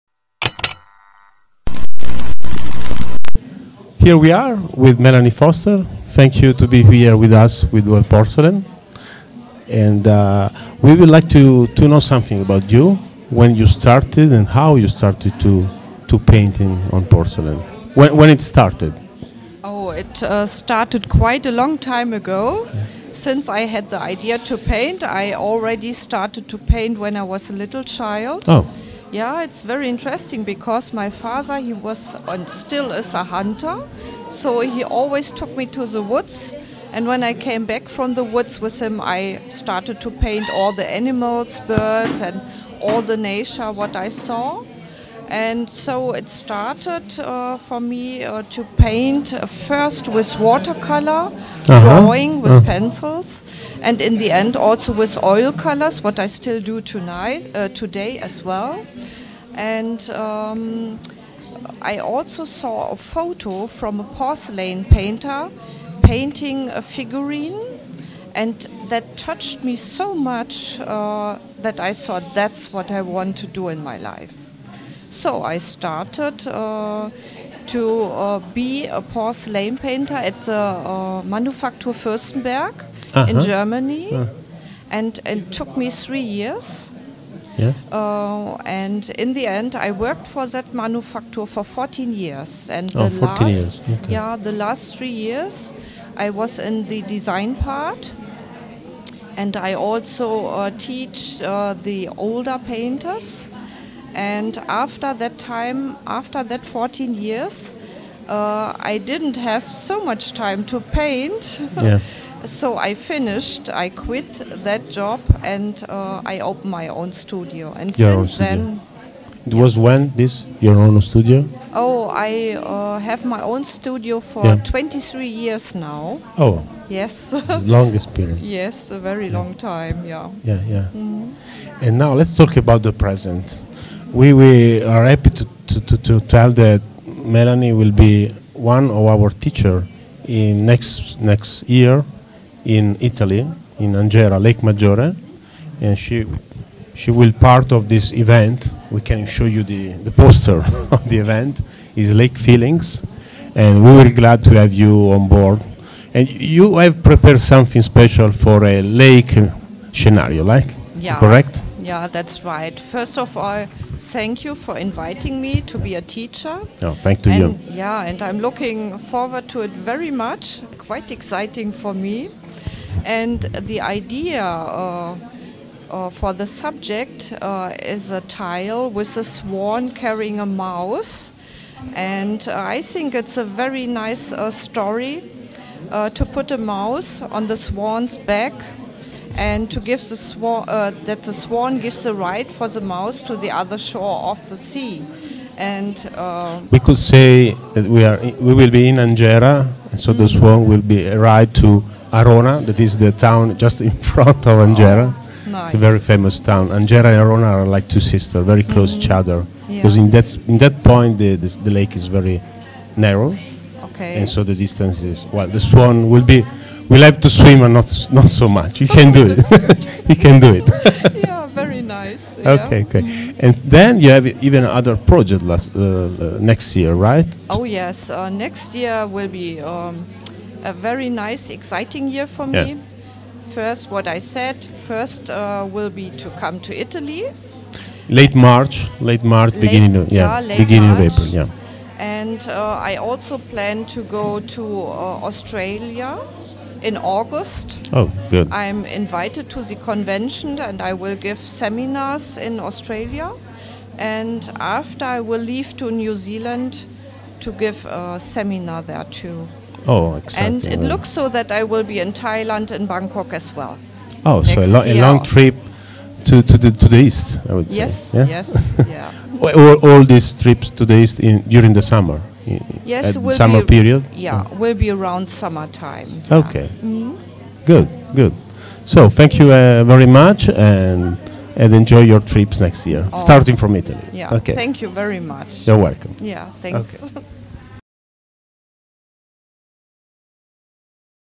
World Porcelain talks with Melanie Foster - Porcelain show 2018, Rapperswill, Switzerland
World Porcelain talks with Melanie Foster about porcelain painting at the World Porcelain stand - Porcelain show 2018, Rapperswill, Switzerland